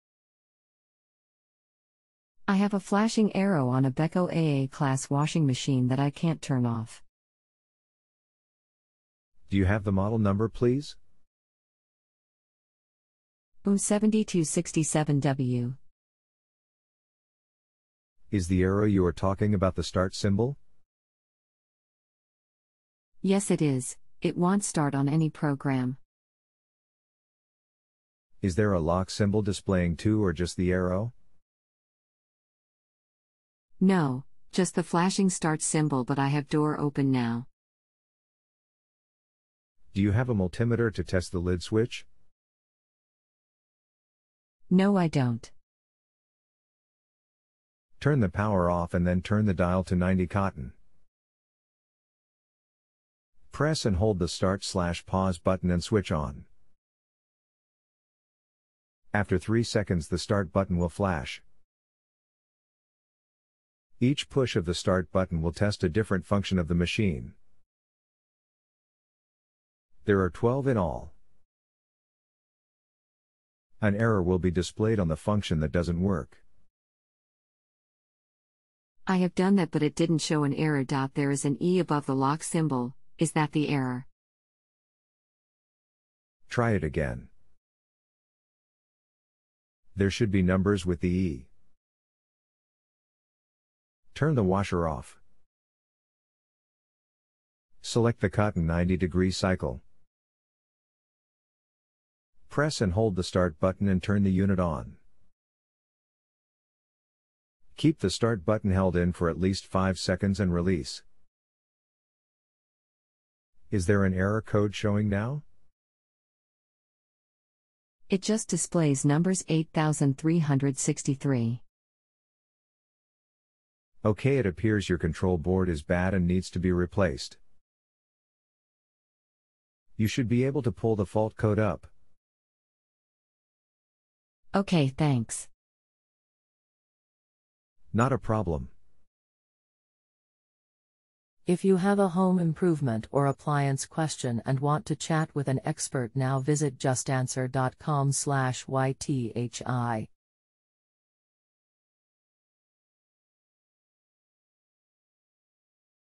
I have a flashing arrow on a beko aa class washing machine
JustAnswer Customer: I have a flashing arrow on a beko aa class washing machine that I cant turn off
AppTech8579: Home Appliance Technician: Do you have the model number please?
JustAnswer Customer: WME7267W
AppTech8579: Home Appliance Technician: Is the arrow you are talking about the start symbol?
JustAnswer Customer: Yes it is,it wont start on any programme
AppTech8579: Home Appliance Technician: Is there a lock symbol displaying too or just the arrow?
JustAnswer Customer: no,just the flashing start symbol but I have door open now
AppTech8579: Home Appliance Technician: Do you have a multimeter to test the lid switch?
JustAnswer Customer: no I dont
AppTech8579: Home Appliance Technician: Turn the power off and then turn the dial to 90 cotton.
AppTech8579: Home Appliance Technician: Press and hold the start/pause button and switch on.
AppTech8579: Home Appliance Technician: After 3 secs the start button will flash.
AppTech8579: Home Appliance Technician: Each push of the start button will test a different function of the machine.
AppTech8579: Home Appliance Technician: There are 12 in all.
AppTech8579: Home Appliance Technician: An error will be displayed on the function that doesn't work.
JustAnswer Customer: I have done that but it didn't show an error.there is an e above the lock symbol,is that the error
AppTech8579: Home Appliance Technician: Try it again.
AppTech8579: Home Appliance Technician: There should be numbers with the E.
AppTech8579: Home Appliance Technician: Turn the washer off.
AppTech8579: Home Appliance Technician: Select the cotton 90 degree cycle.
AppTech8579: Home Appliance Technician: Press and hold the start button and turn the unit on.
AppTech8579: Home Appliance Technician: Keep the start button held in for at least 5 seconds and release.
AppTech8579: Home Appliance Technician: Is there an error code showing now?
JustAnswer Customer: it just displays numbers 8363
AppTech8579: Home Appliance Technician: Okay it appears your control board is bad and needs to be replaced.
AppTech8579: Home Appliance Technician: You should be able to pull the fault code up.
JustAnswer Customer: ok thanks
AppTech8579: Home Appliance Technician: Not a problem.